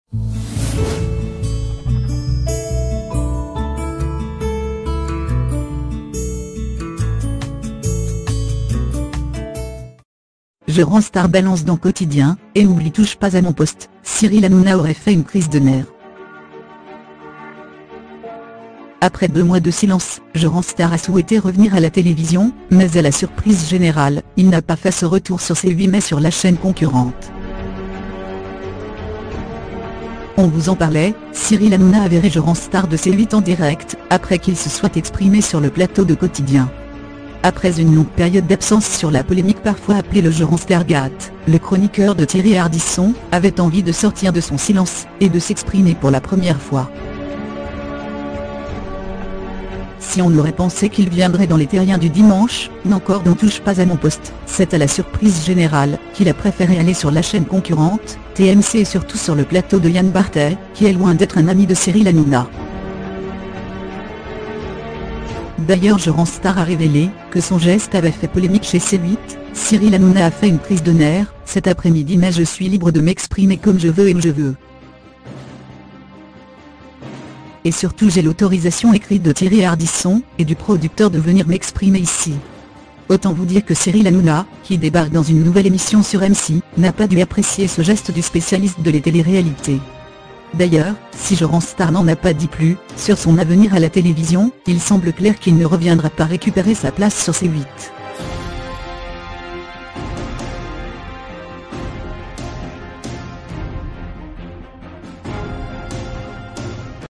Jeremstar balance dans Quotidien et oublie TPMP, Cyril Hanouna aurait fait "une crise de nerfs"
Jeremstar balance dans Quotidien et oublie Touche Pas à Mon Poste, Cyril Hanouna aurait fait "une crise de nerfs"
Après deux mois de silence, Jeremstar a souhaité revenir à la télévision, mais à la surprise générale il n'a pas fait ce retour sur C8 mais sur la chaine concurrente...